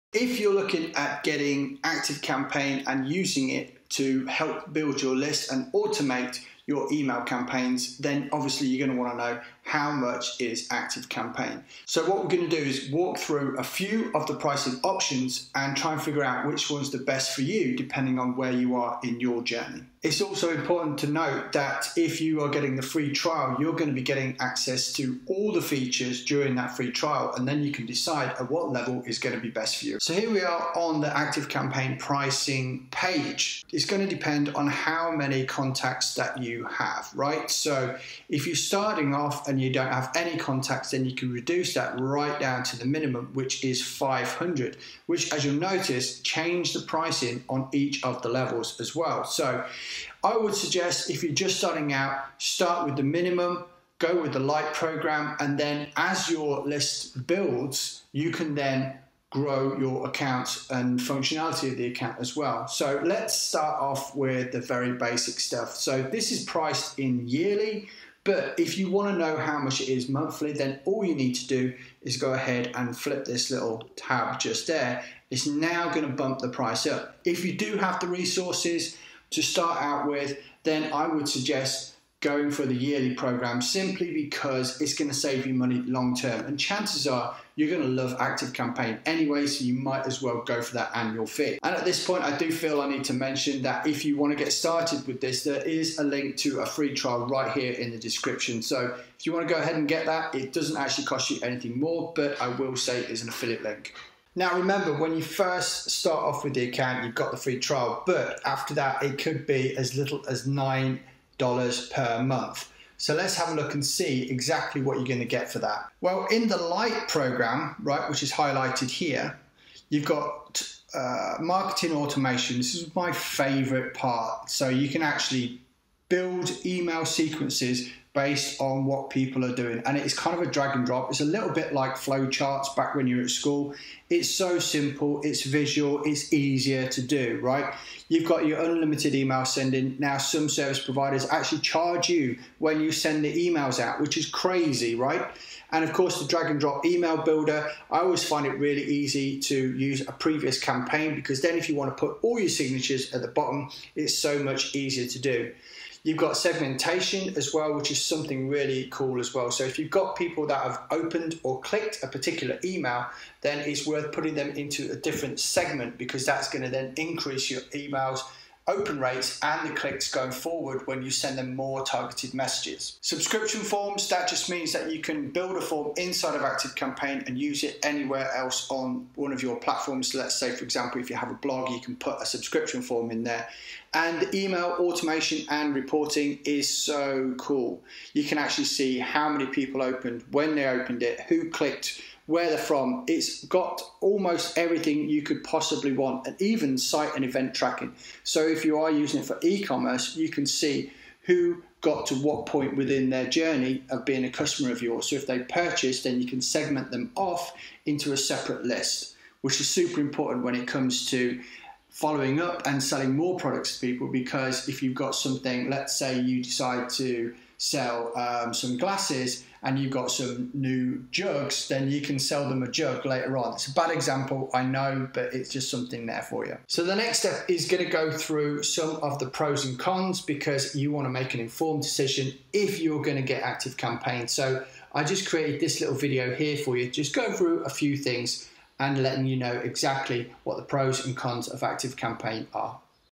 ActiveCampaign Pricing Made Simple. It's More Affordable Than You Think
Activecampaign pricing starts at just $9 per month.

That's going to get you allow you to build your contact list up to 500 people.

and still get's you full access to :
Marketing Automation

Unlimited Email Sending

Drag & Drop Email Builder

Segmentation

Subscription Forms

Email & automation reporting

Site and Event Tracking

Dot Com Secrets Book ( free with a few $'s shipping )

Resources :
- start the 3 day business breakthrough challenge
- Get a FREE Clickfunnels trial + my special bonuses.